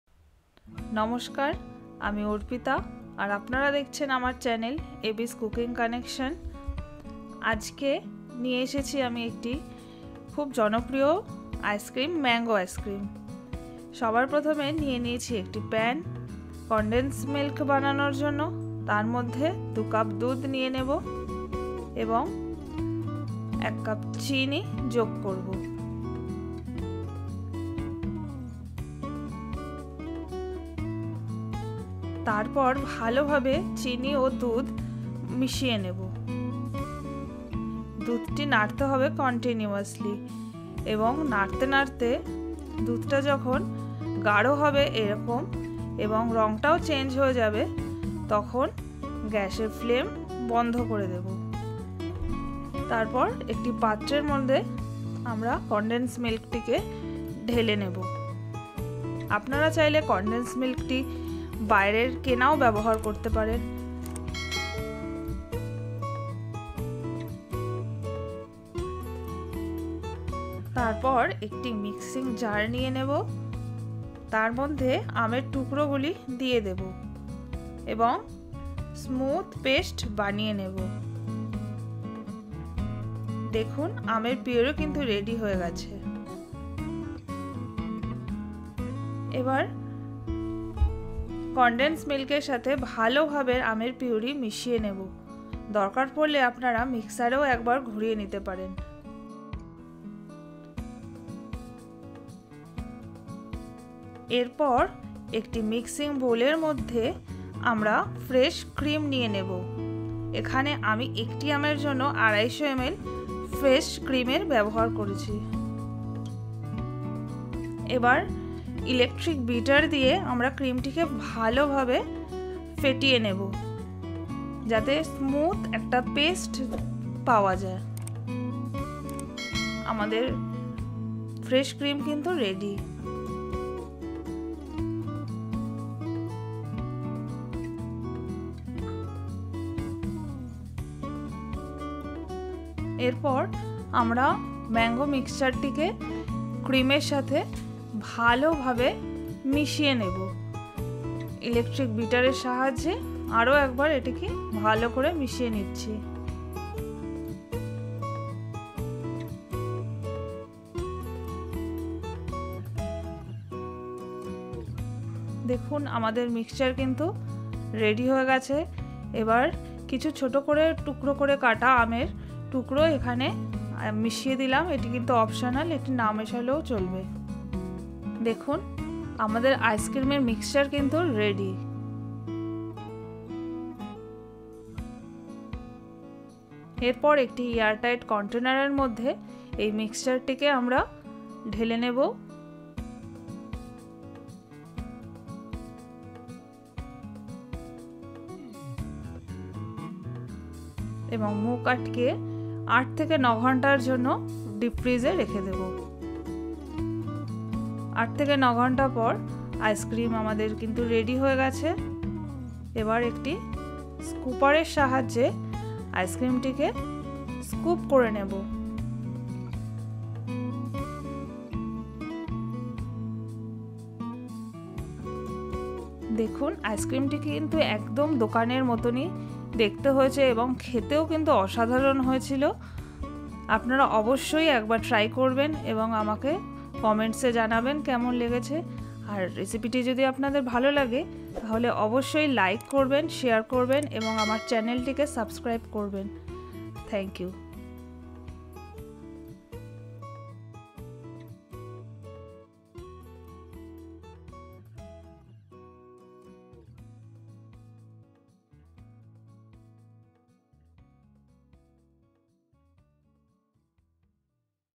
mango ice cream recipe | mango ice cream at home | পাকা আমের আইসক্রিম রেসিপি
mango ice cream recipe | mango ice cream recipe | mango ice cream at home | পাকা আমের আইসক্রিম রেসিপি | ESP23
Ingredient of mango ice cream
1) Mango – 1 (big size).
2) Milk – 2 cup.
3) Sugar -1 cup.
4) Fresh cream – 250 ml.

Your queries -